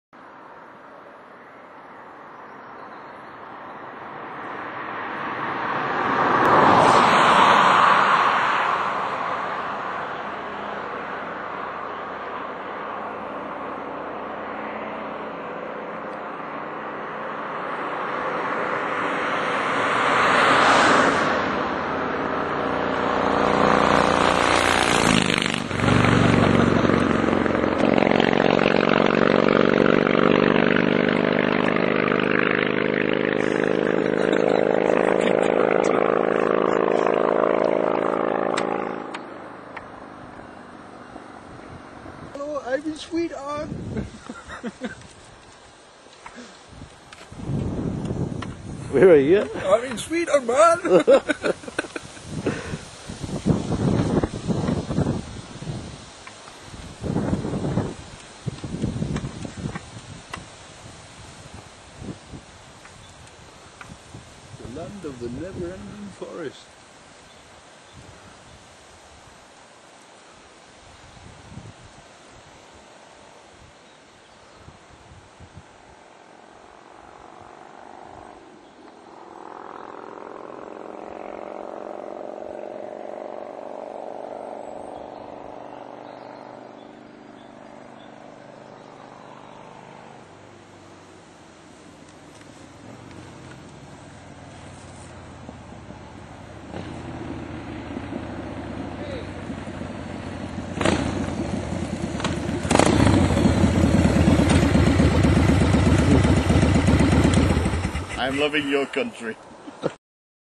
harley in sweden
riding on the swedish roads